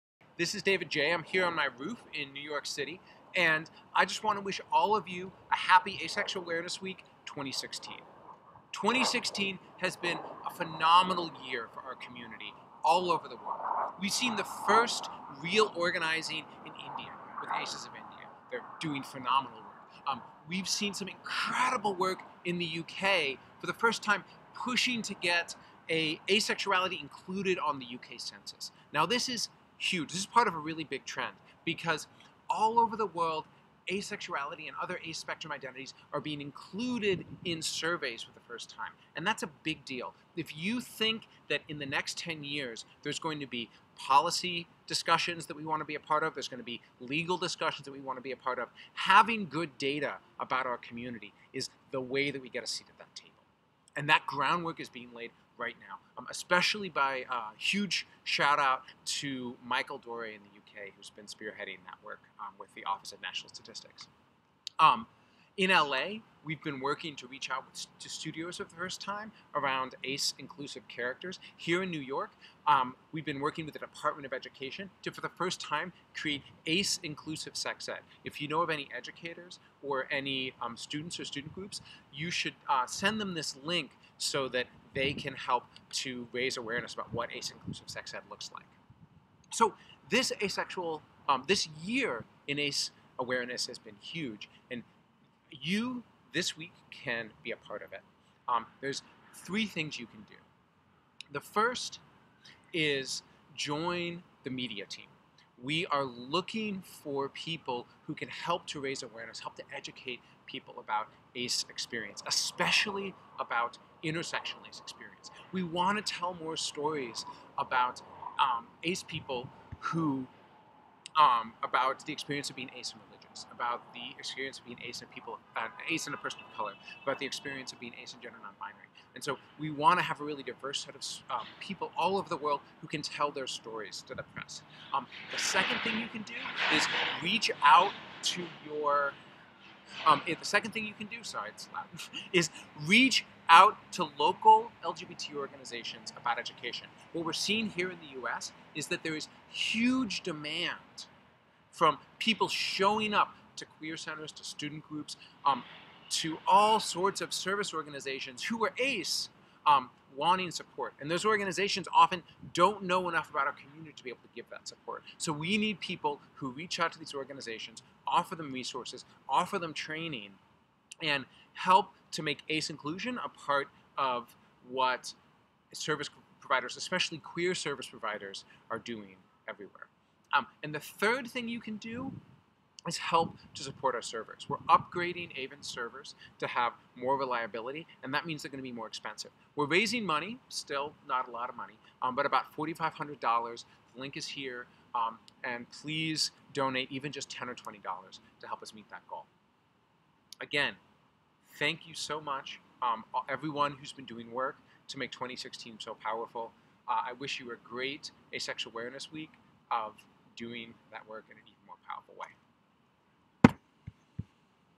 Asexual Awareness Week 2016 Update
Happy AAW2016. It's been a big year for the ace community, here's how you can support ace awareness!